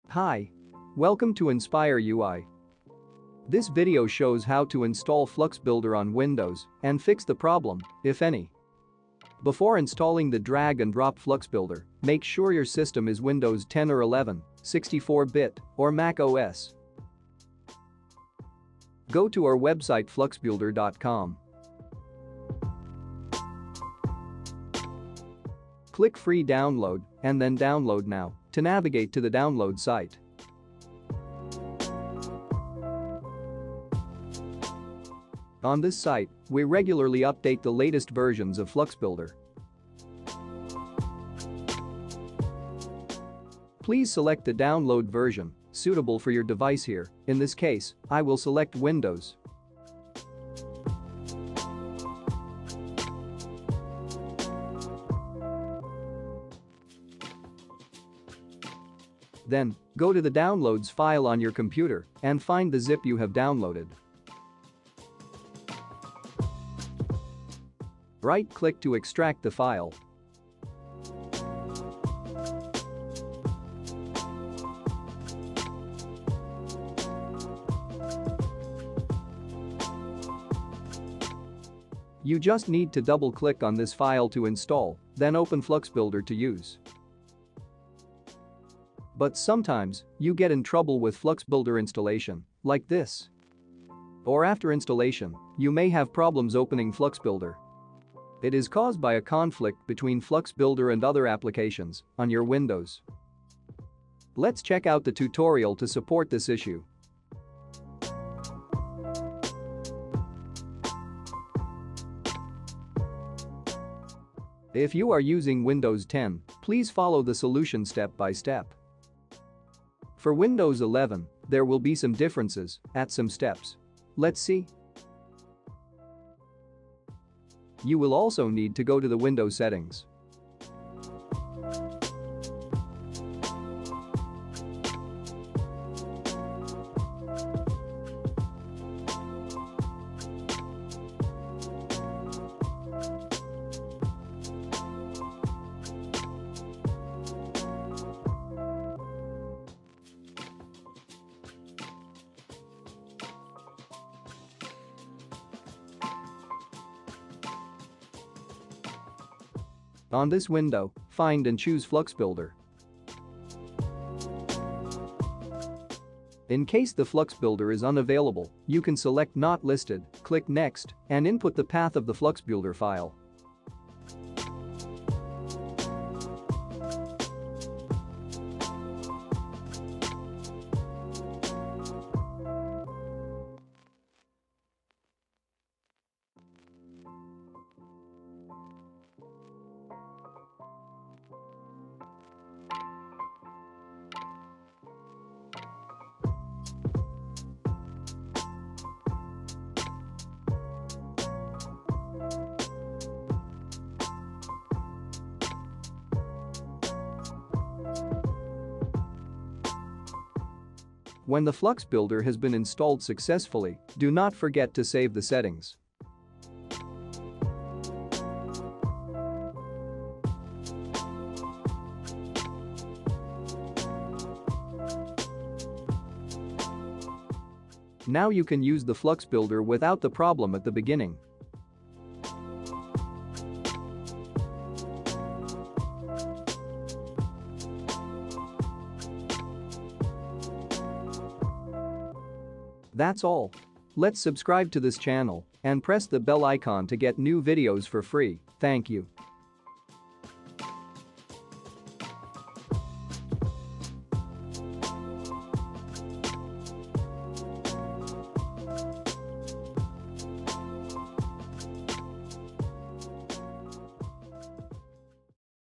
FluxBuilder: Install FluxBuilder on Windows (Best No-code Flutter App Builder)
Learn how to install the drag & drop FluxBuilder on Windows and resolve issue (if any). Please note that FluxBuilder 1.4.1 has just updated on 16 Feb 2022.

FluxBuilder:
💌 The drag&drop FluxBuilder tool creates mobile Apps for your ecommerce / wordpress Websites on both iOS and Android.
💌 No need to buy FluxBuilder. To activate license, just input your purchase code of FluxStore apps
💌 Do not test the App's features on the simulator of FluxBuilder. Please test features of app on “Demo" in

Thank you so much and we look forward to serving you!